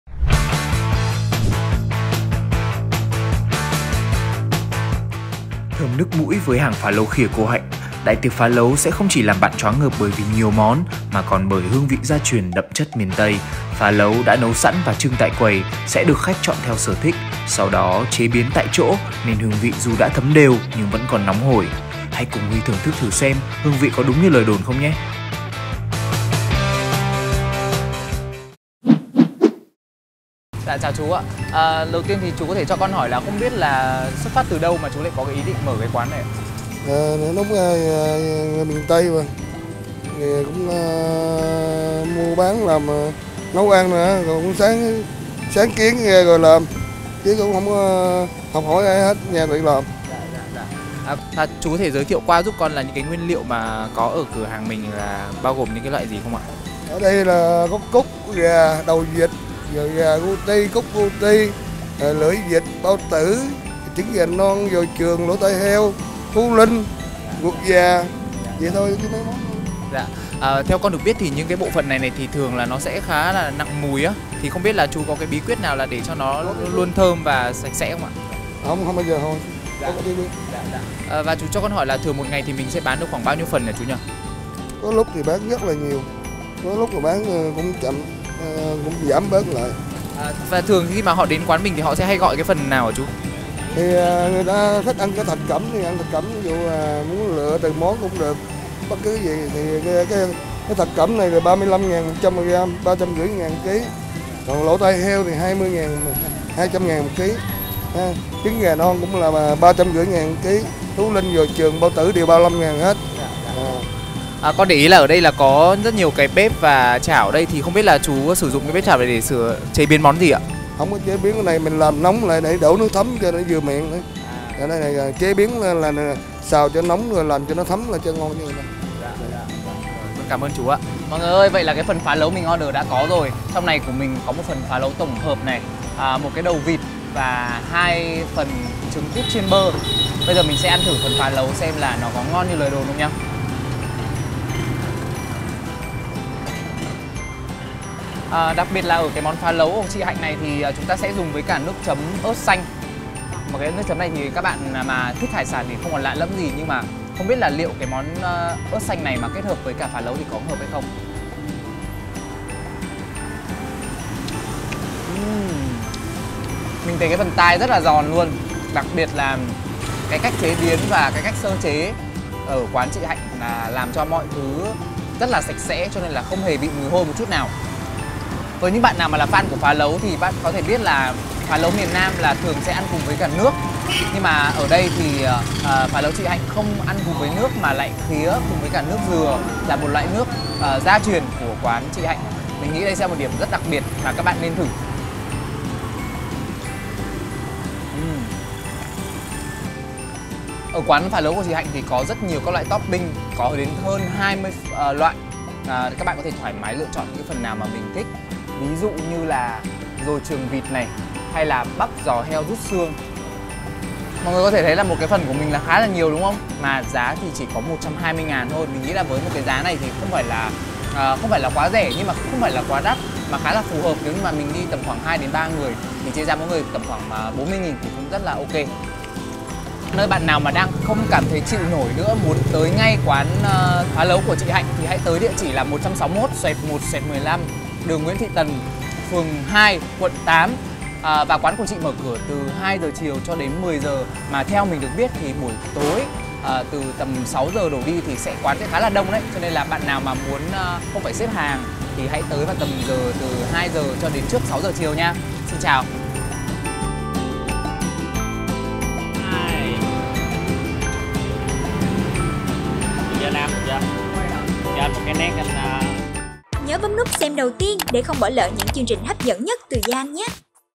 ĐẠI TIỆC PHÁ LẤU KHÌA NƯỚC DỪA 12 MÓN THƠM BANH MŨI
★ | Đi đâu đây - ĐẠI TIỆC PHÁ LẤU KHÌA NƯỚC DỪA 12 MÓN THƠM BANH MŨI ✔

Hãy theo dõi Yan trên:
✔️Youtube Yan News :